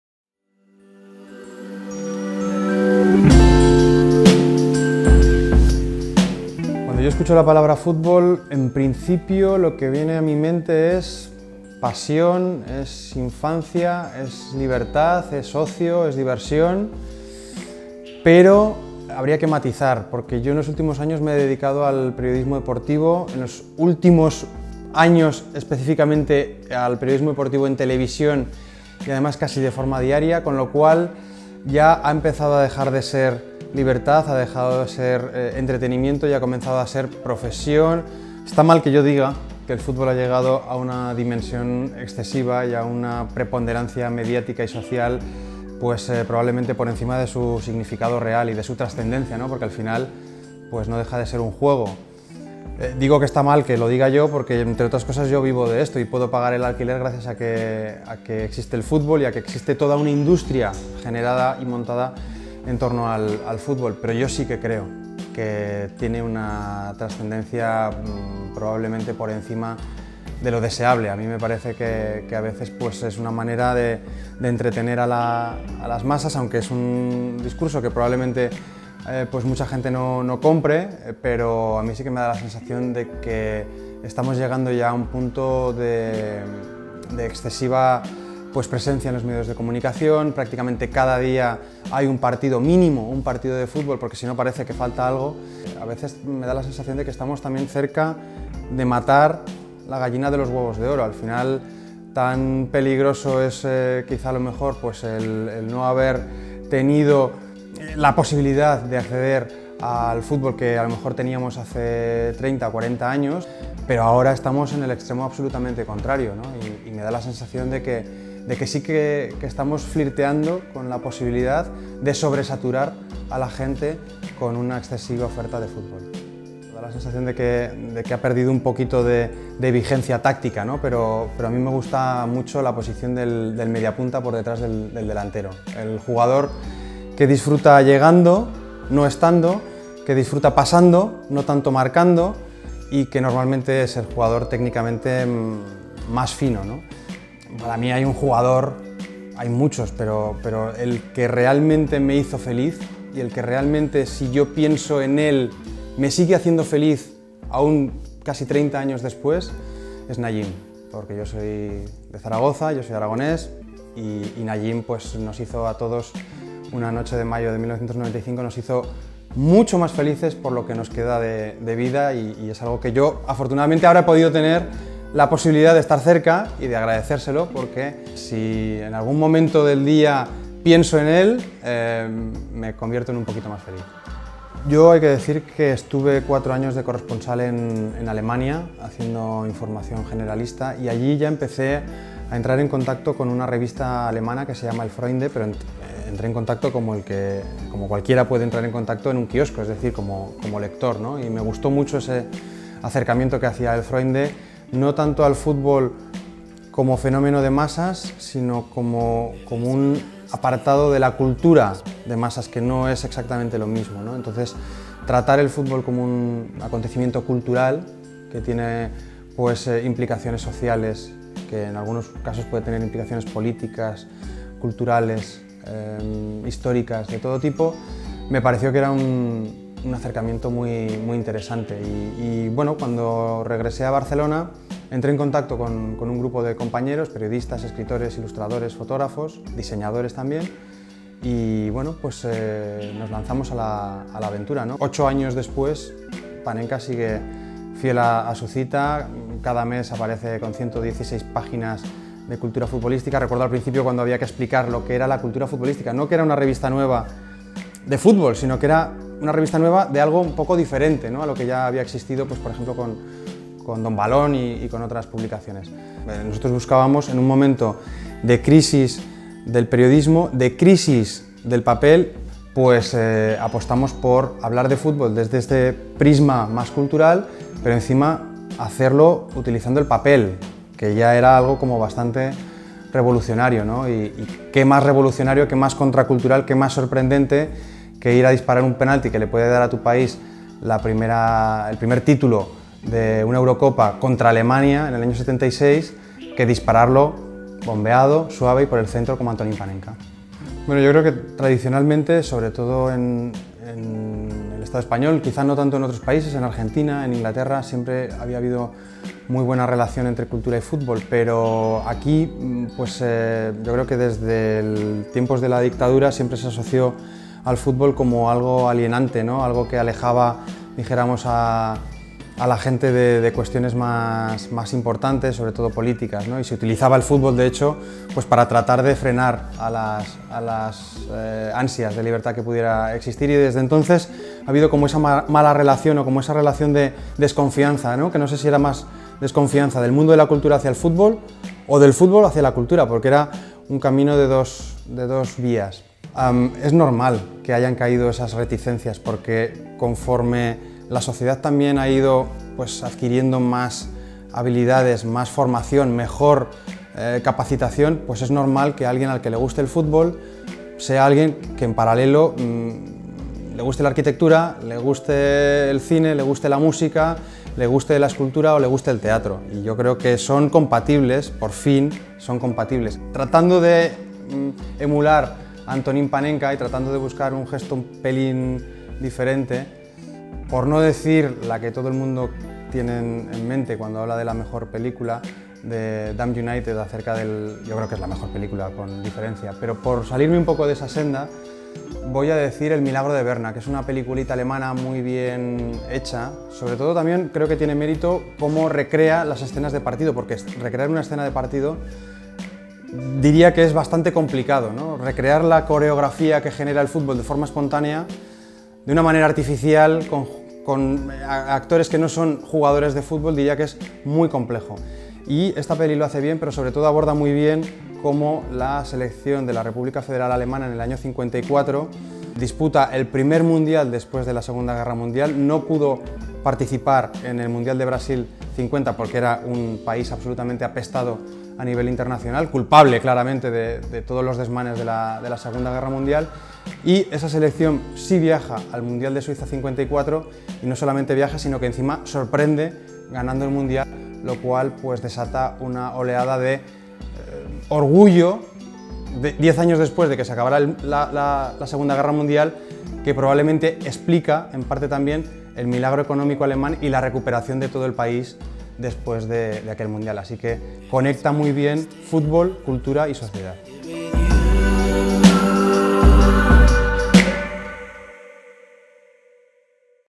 Cuestionario Korner 2019: Aitor Lagunas
Korner 2019. 5 de febrero, San Sebastián.

Aitor Lagunas, historiador y periodista. Nos ha acompañado en esta edición como moderador en el Derbi de Leyendas con Luis Arconada y José Ángel Iribar. Ahora, comparte en Cuestionario Korner su experiencia y opiniones sobre la cultura y el fútbol.